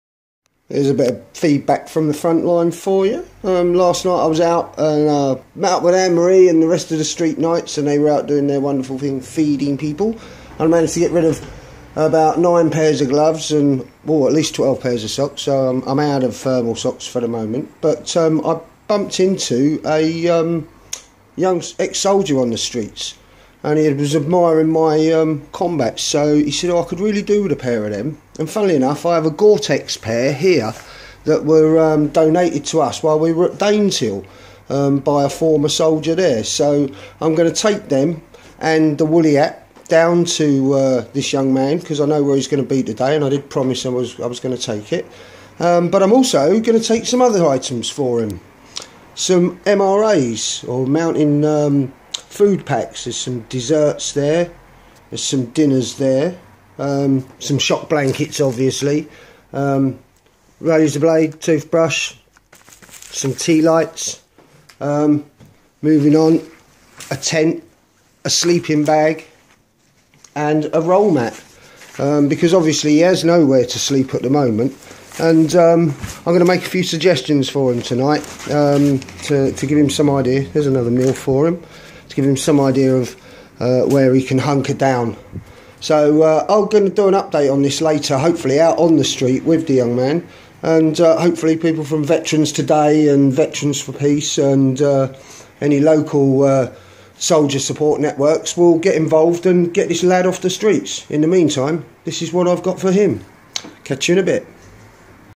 OpSafeWinter. 2016 Nottingham's Homeless Ex Forces
Whilst out with the StreetKnights feeding the homeless last night. i met a young lad, ex services who is sleeping on the street of NOTTINGHAM.
He remarked upon my combat trousers, summer issue, and lamented how he wished he had some again.
I knew I could do something about that the next day, so I did.